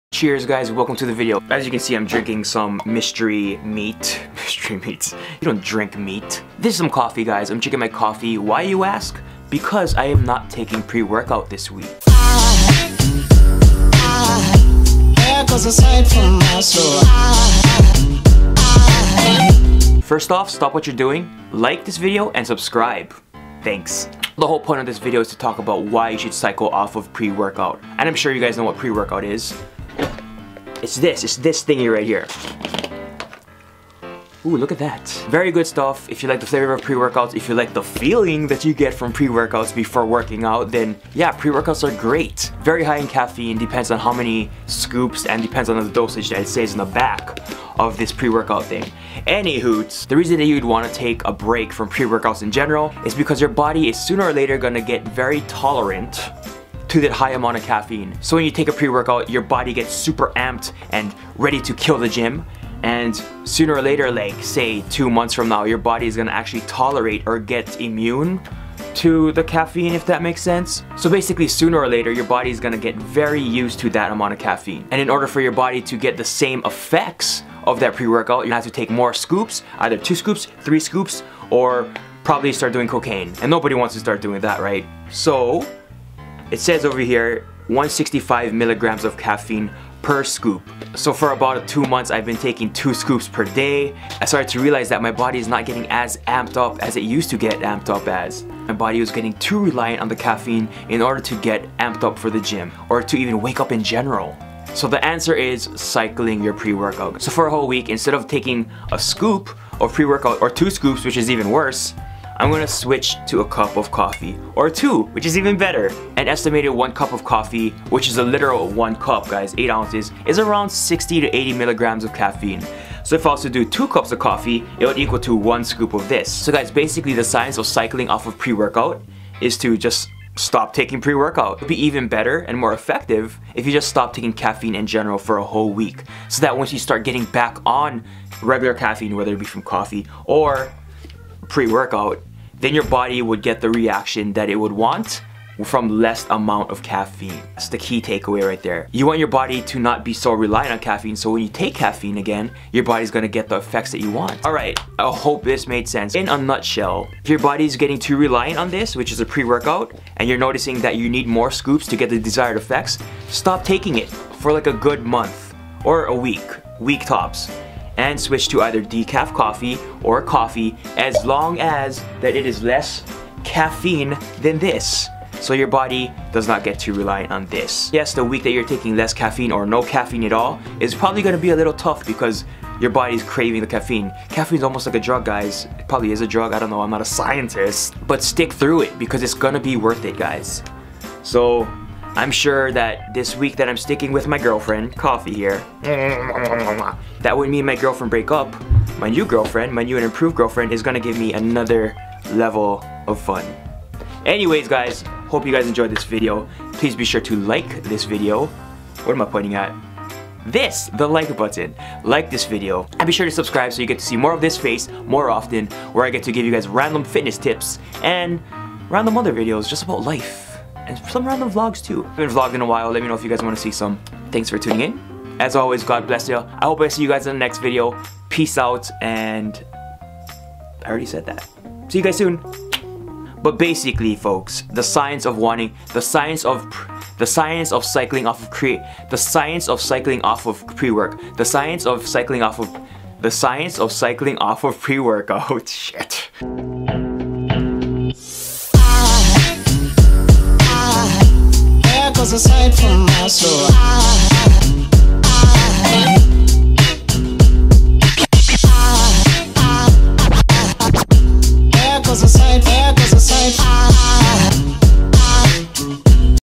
HOW TO TAKE PRE WORKOUT + CYCLE PRE WORKOUT
Learn how to take pre workout and mainly how to cycle pre workout in this video!

Hey Fam!
So in this video we basically go over how I cycle off pre workout and why you should consider cycling off and on pre workout. In a nut shell we want to cycle off pre workouts or caffeinated  products like monster, red bull, or coffee so that our body doesn't get reliant on it! You'll notice that through time of not taking a pre workout when you finally do take it again you'll recieve massive amounts of energy like it was the first time taking it lol. Well i hope this video was helpful.